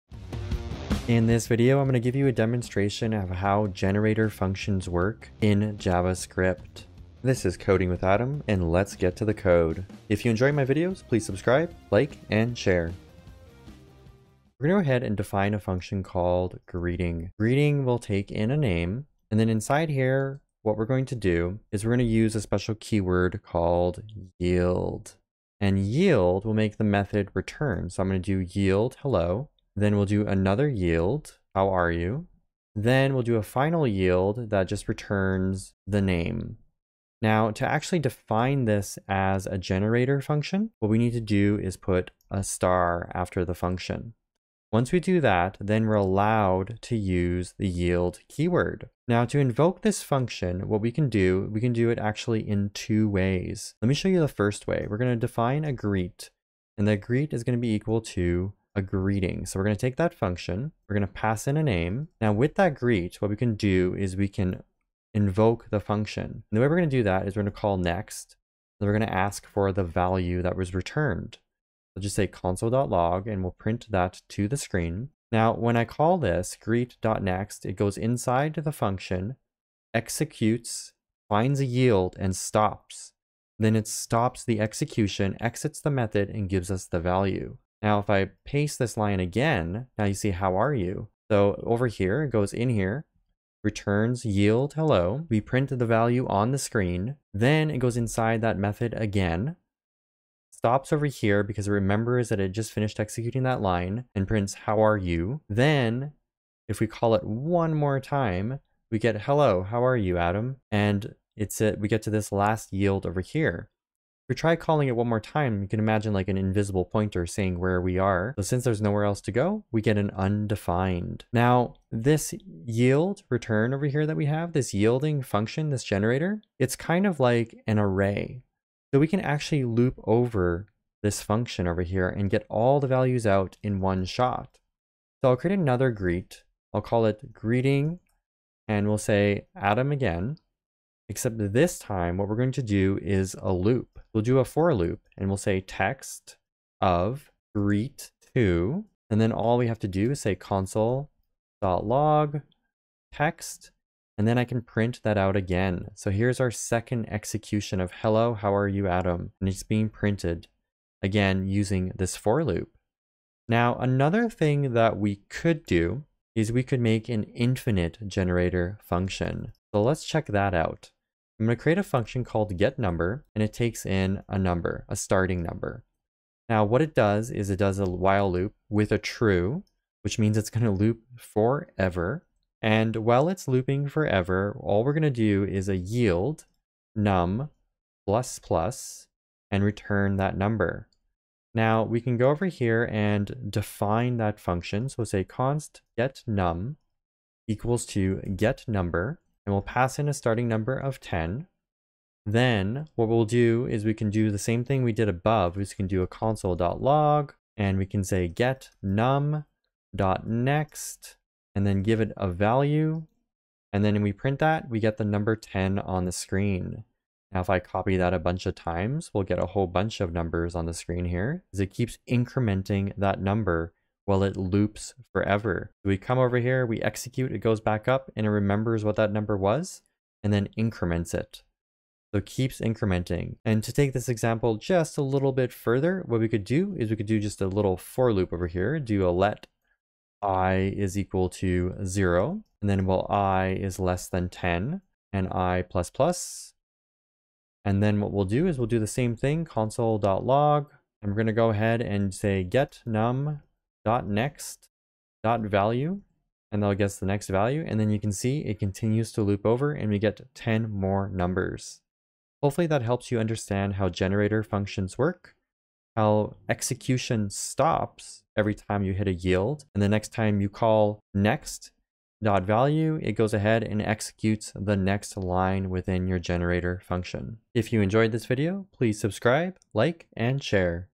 Learn JavaScript Generators Quickly
In this video we learn about JavaScript Generator functions.
This is a great introduction to generator functions. Generators are functions that can be exited and later re-entered.

You will learn what the yield key word does. How a generator function suspends execution when yield is called also known as deferred execution. You can do fun things like make an infinite number generator.